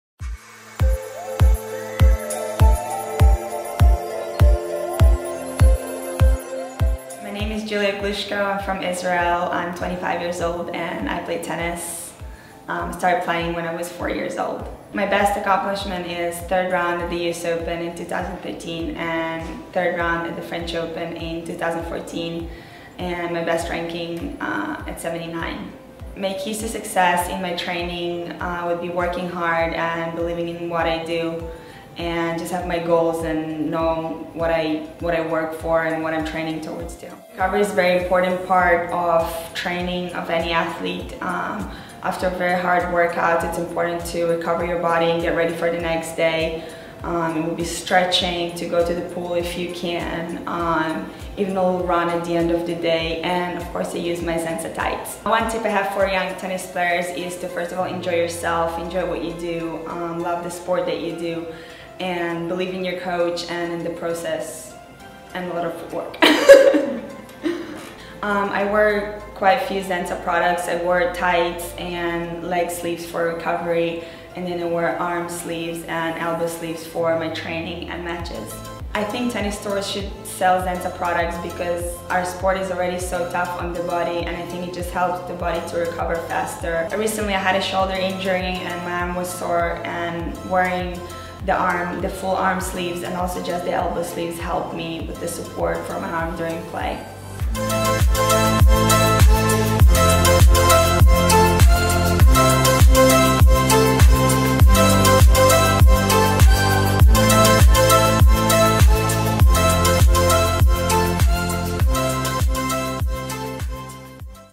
WTA Professional Tennis Player Julia Glushko Chooses Zensah
WTA Professional Tennis Player Julia Glushko uses Zensah as part of her training, for recovery, and during matches. Julia uses the Zensah Compression Elbow Sleeve to help relieve tennis elbow pain, the compression leg sleeves and compression tights to improve muscle recovery, and the arm sleeves for muscle stabilization.